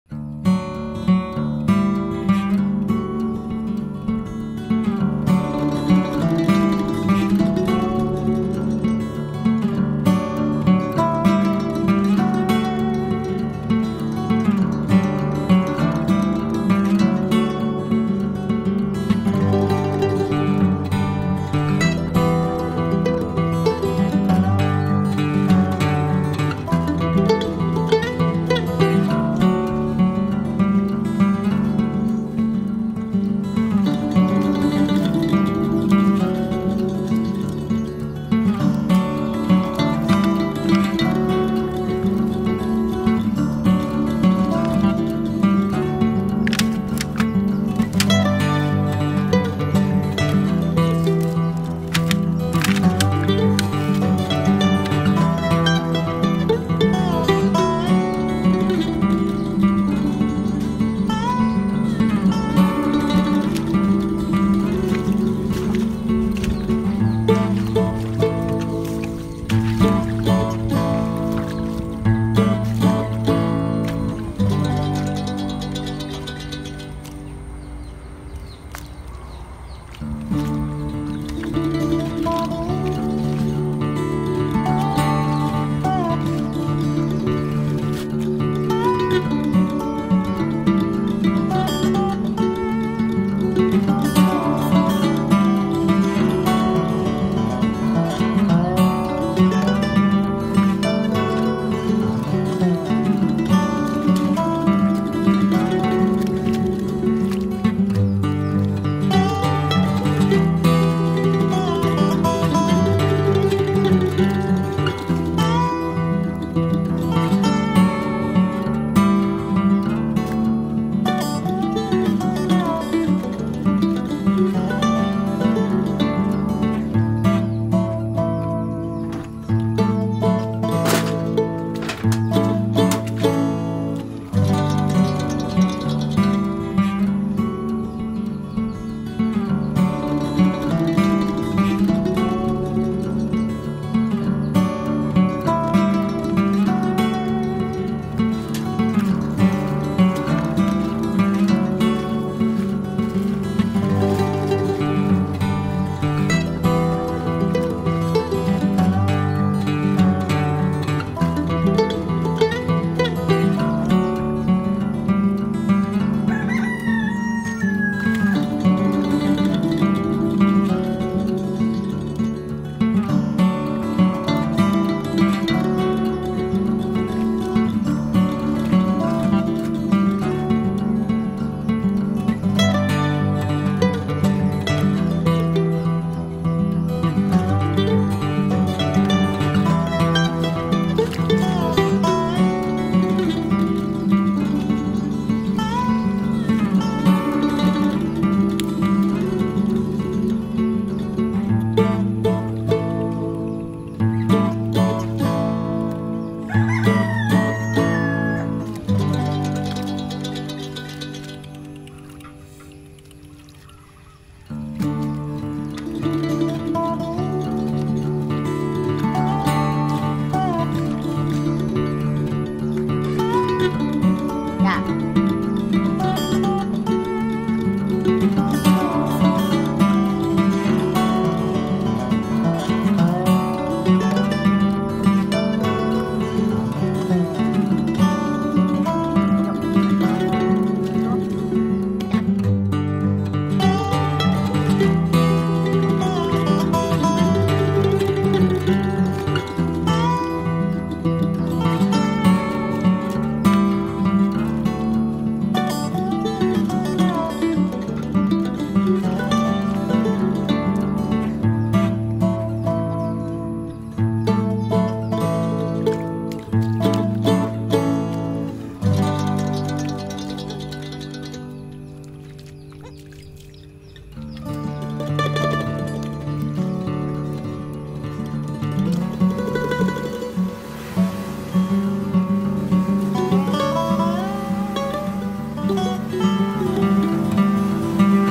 The life of an orphan who catches fish for cooking
Because of life, this boy went fishing in the water like this, no one wants to do this, he is a child living in Cambodia, he is 10 years old, he has only two siblings and the only child is a younger brother She has been orphaned by her parents since she was 6 years old and she is still studying. Because of this kind of life, I took her with me to make a video, and this is her first video.
Please support and encourage this orphan. I will make another video with this boy. Of this orphan.